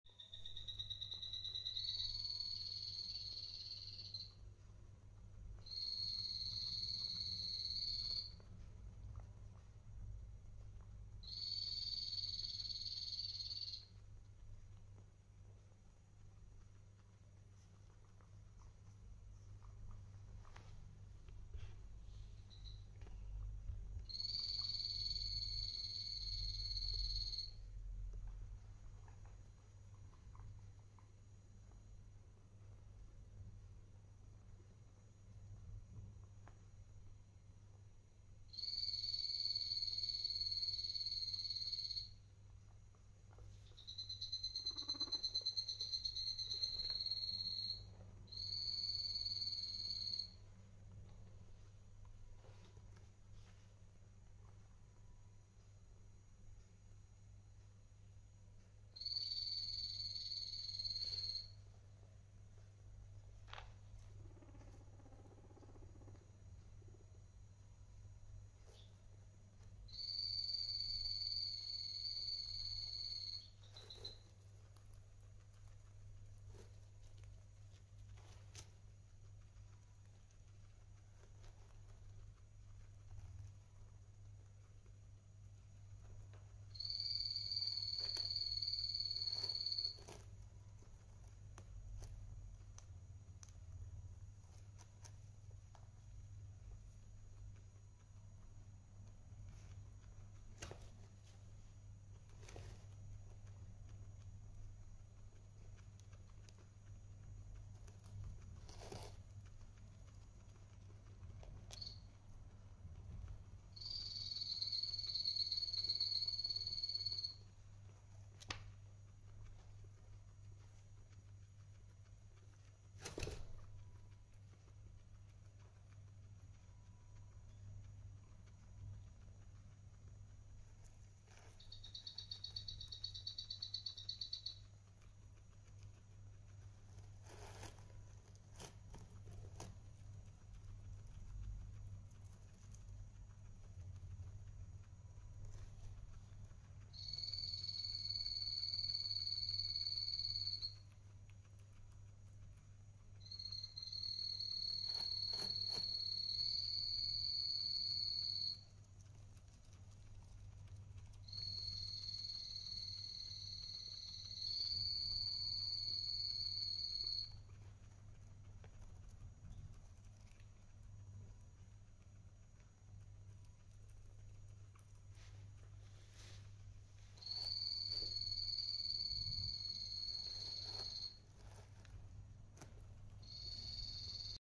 Сutest Guinea pigs | Guinea Pig Day 6.12.2022. 2
Сutest Guinea pigs.
These are our Guinea pigs. We adopted them as adults.